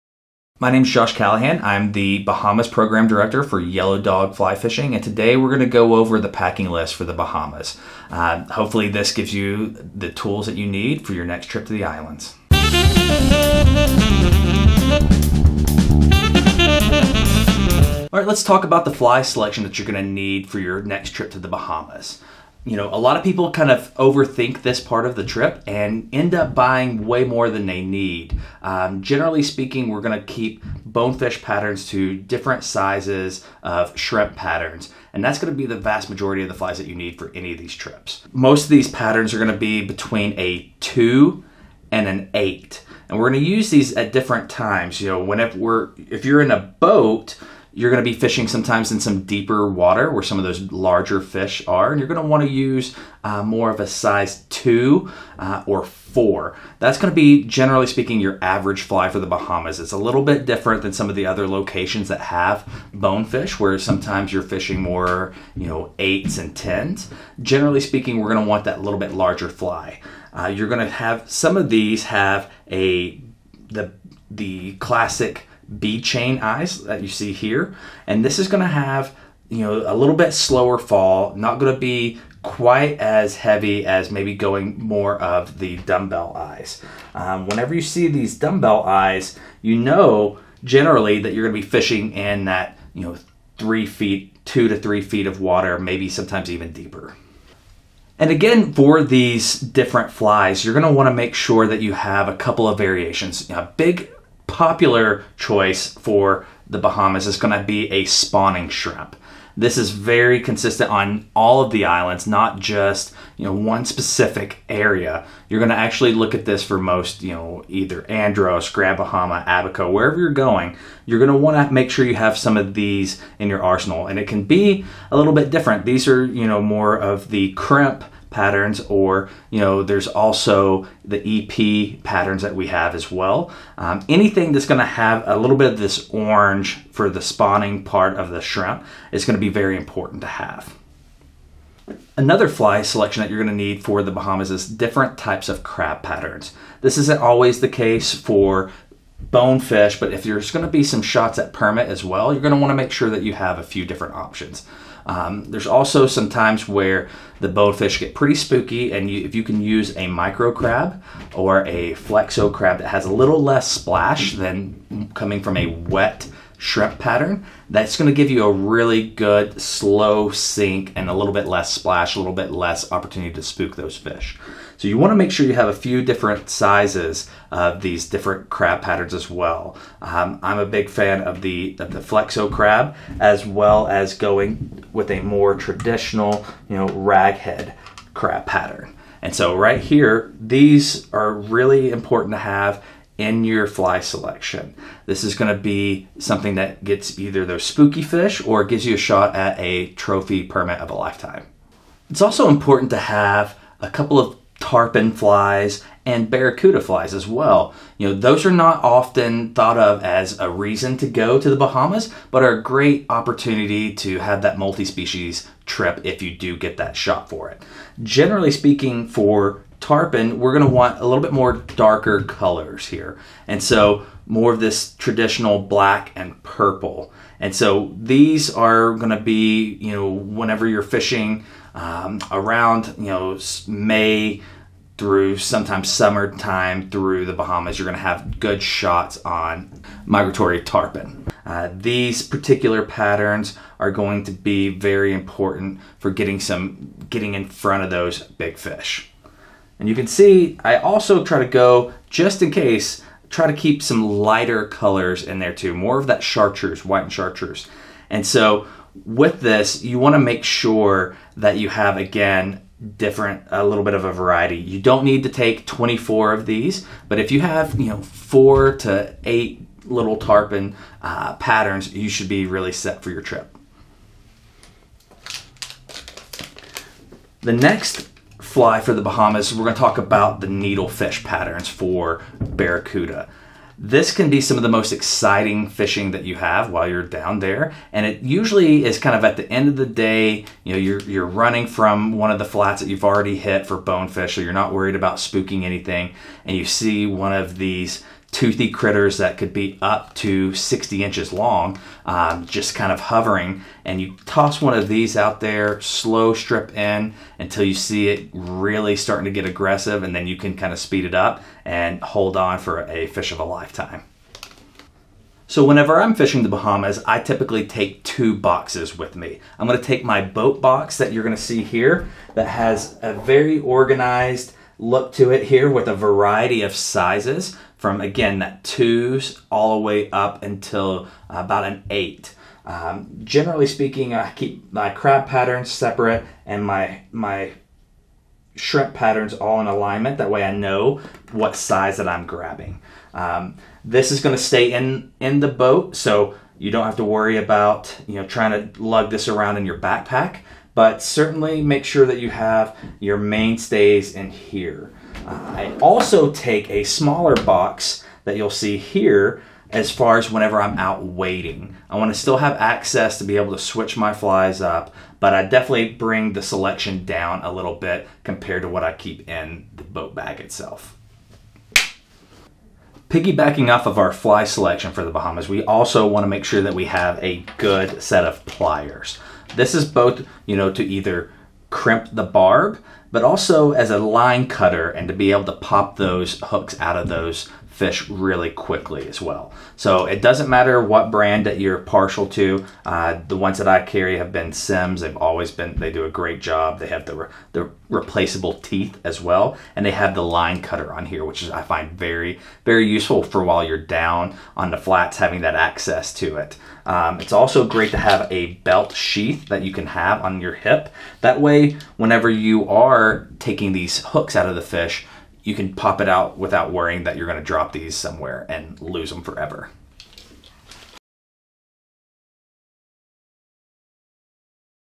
Essential Flies for Fly Fishing in The Bahamas
The Bahamas: A bonefish paradise. The collection of islands is home to likely the greatest bonefishing in the Western Hemisphere, and its proximity to the United States makes it easy to access for many travelers. Flies for the Bahamian islands do not need to be complicated, and Josh Callihan walks us through the key components of a good fly selection for bonefish throughout the Bahamas.

Additionally, he discusses options for permit, tarpon, and barracuda should those opportunities present themselves, as some areas of the Bahamian islands have an excellent reputation for larger game species.